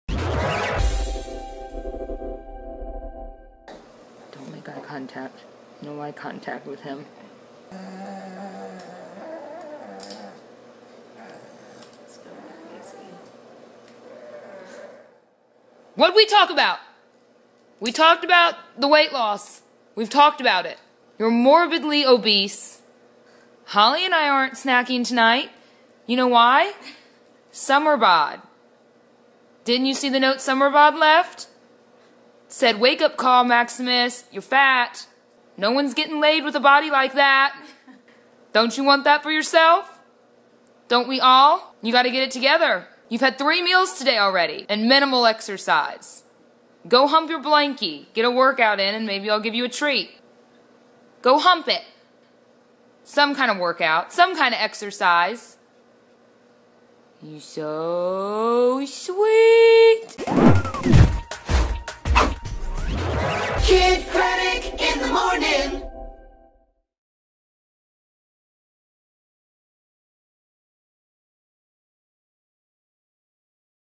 Jenna's dog doesn't care about his Summer bod
Jenna's dog, Maximus, lets everyone know when he is hungry.